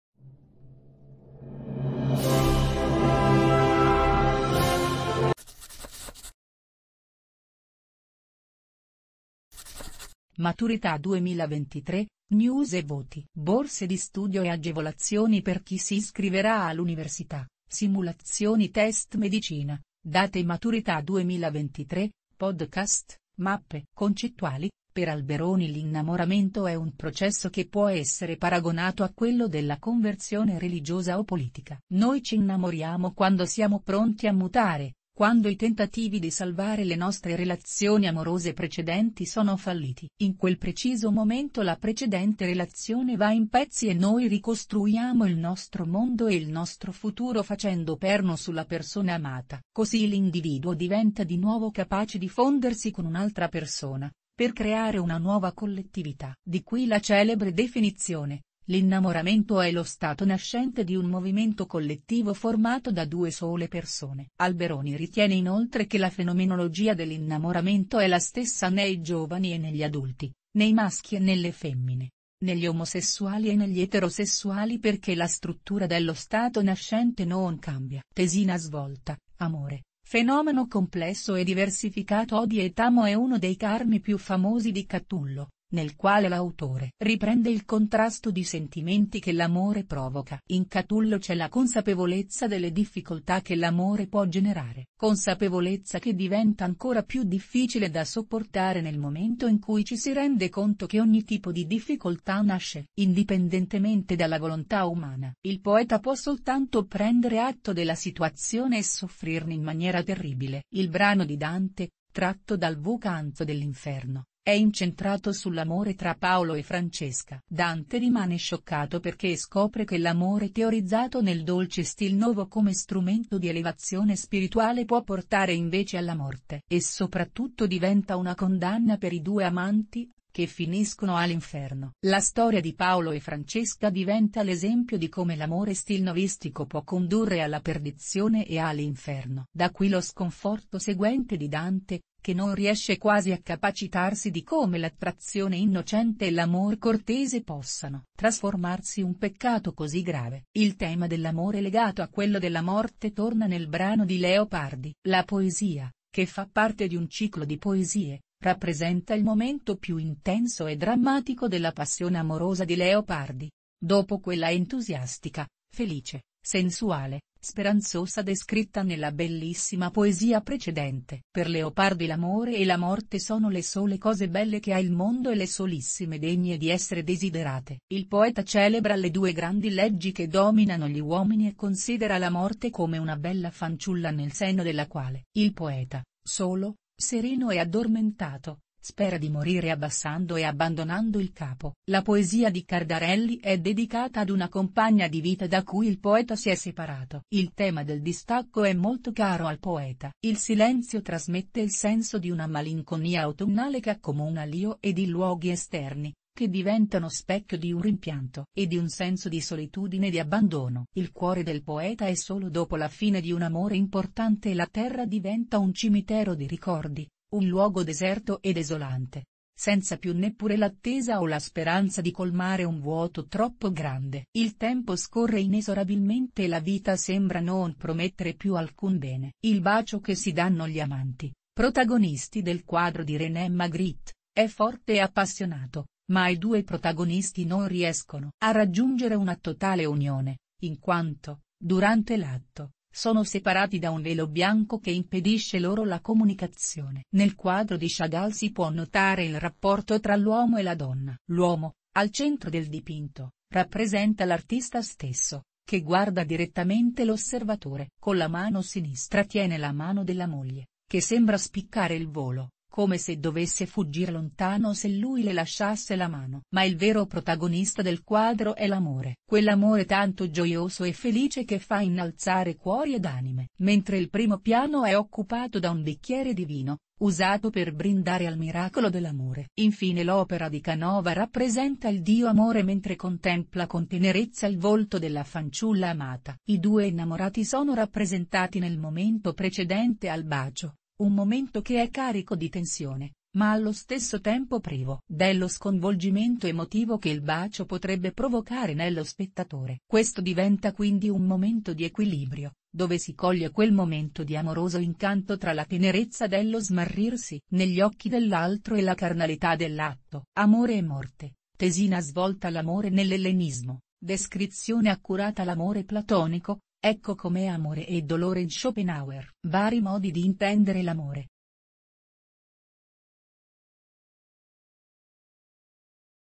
Innamoramento e amore tra letteratura e arte
Spunti di riflessione della tipologia B, saggio breve o articolo di giornale, ambito artistico-letterario, per la prima prova della maturità 2009